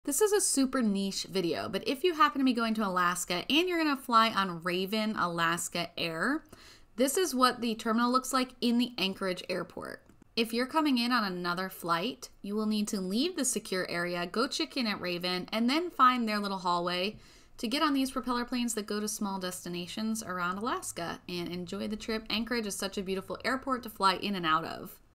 What to Expect on a Ravn Air Flight in Alaska
This is for my fellow type A people that are searching Ravn Air terminal on Google maps in Anchorage. It's in the same airport but outside of the secure area.

What small airports are you flying into in Alaska?

Independent Agent of Cruises & Tours Unlimited
FST No. ST15578, CST No. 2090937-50, WA UBID No. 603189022, IA Registered Agency No. 1202
🕐 We save you time so that you can focus on making lifelong memories.